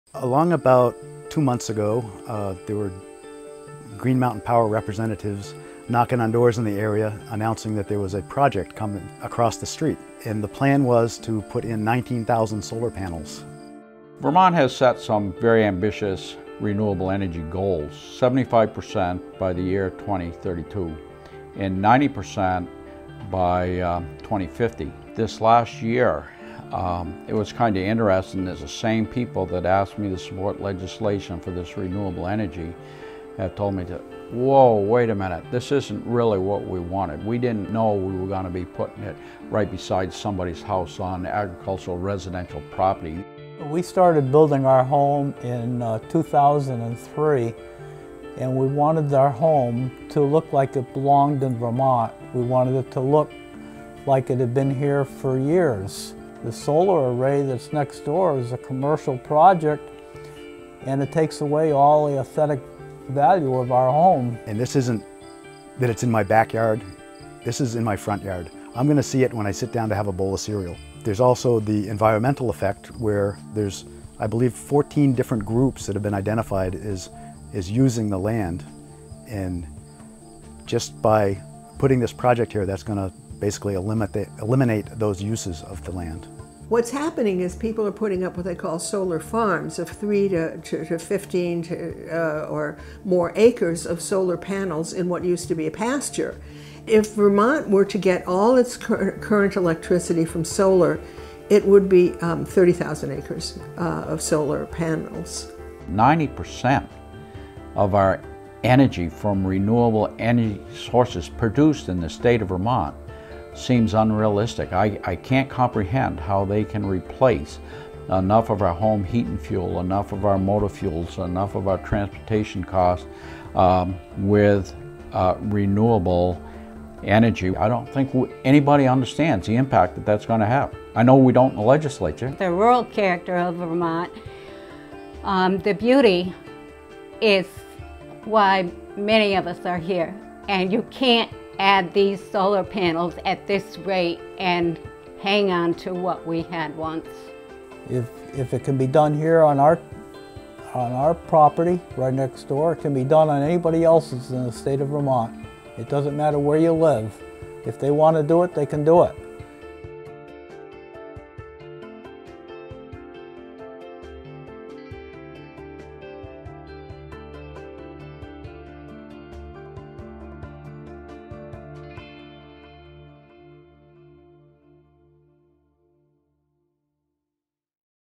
Renewable Energy Consequences in Vermont
With the passage of Act 56 by the Vermont Legislature, solar arrays are appearing throughout the state. Here's how it's impacting Vermonters.